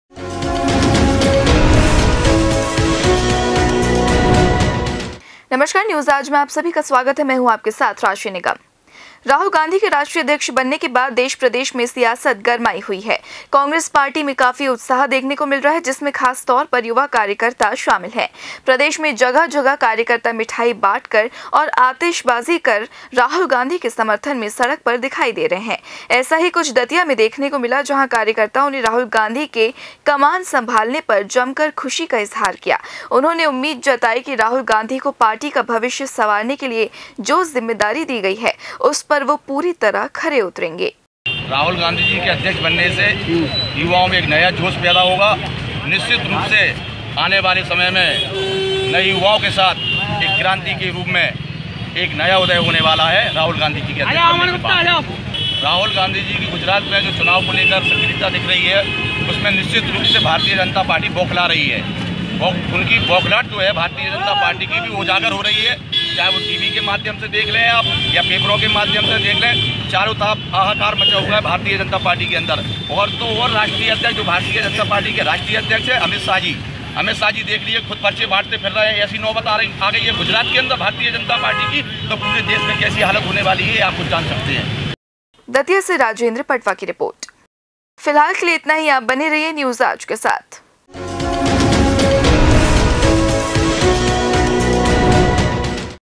राहुल गांधी के राष्ट्रीय अध्यक्ष बनने से युवाओं में जोश
मिठाइयां और आतिशबाजी कर मनाई खुशीयां